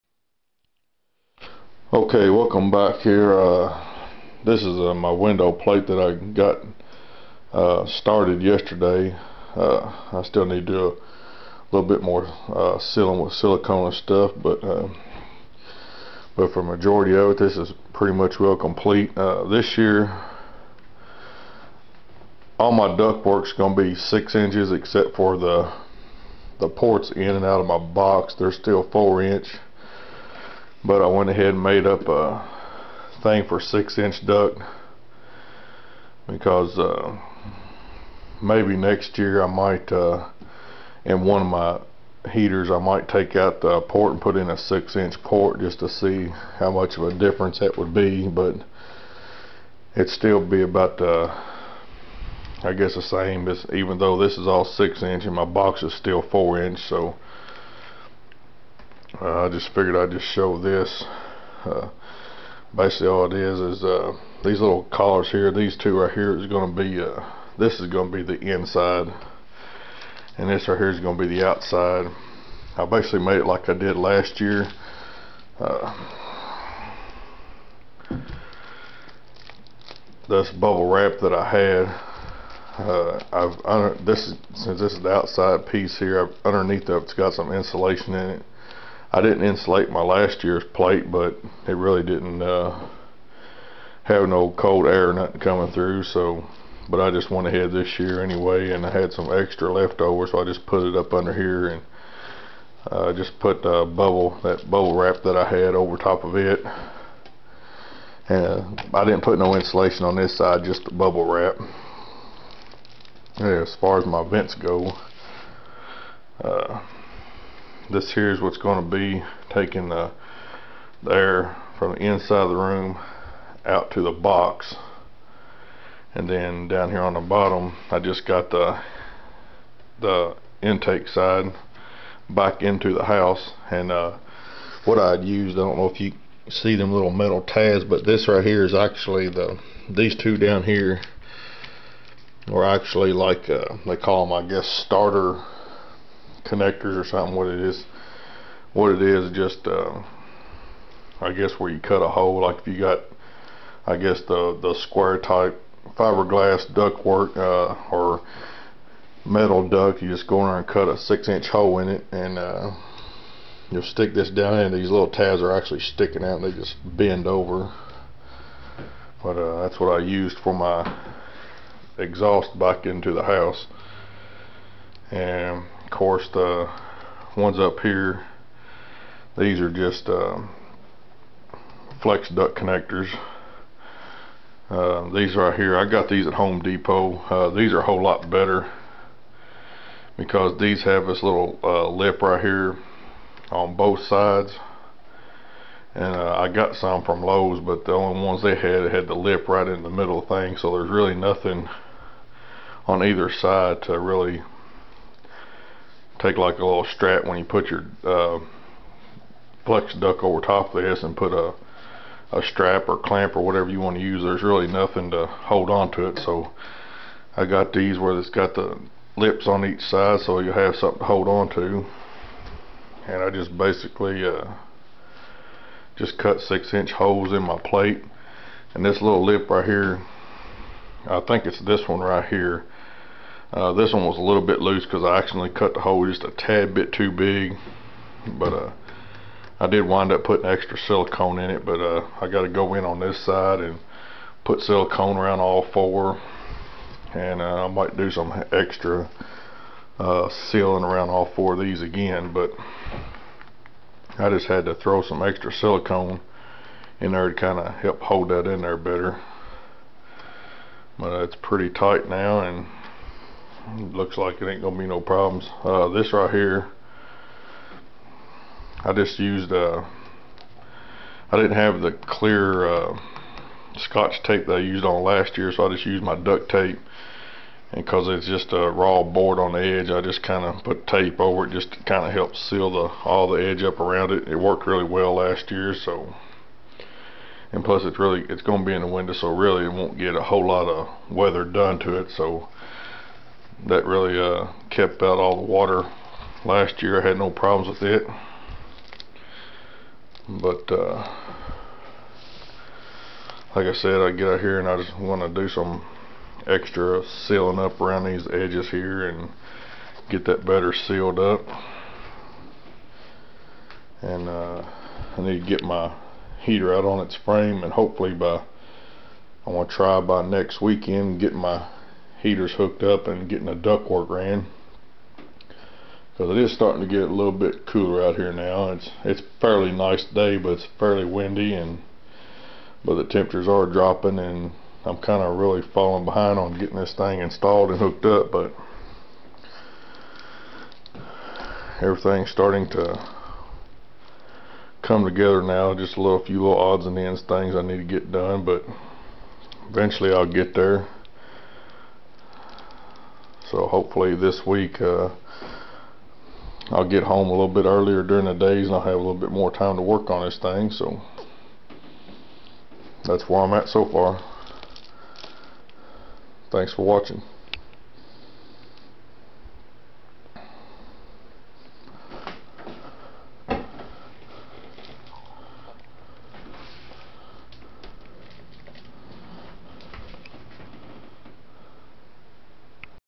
Solar Heater #2 Build #22.MOV
This is the window plate for my two sodapop solar heaters. This way I can test both at the same time side by side to see the temp difference.